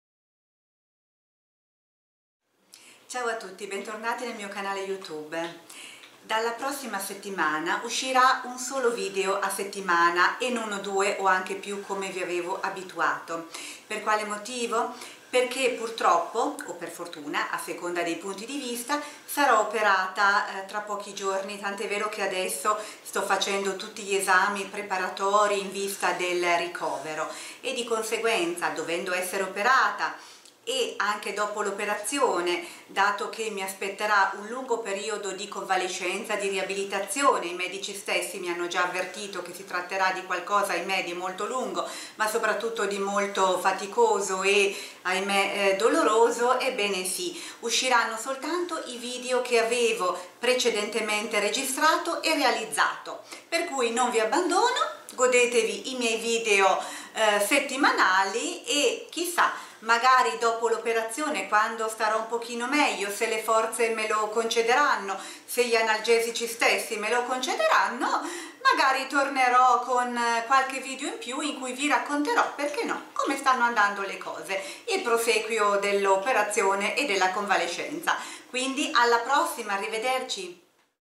A breve sarò operata!
A breve sarò operata...ma tranquilli, non vi abbandono!
Usciranno comunque  i video precedentemente registrati, uno a settimana.
Nella community, la scaletta aggiornata.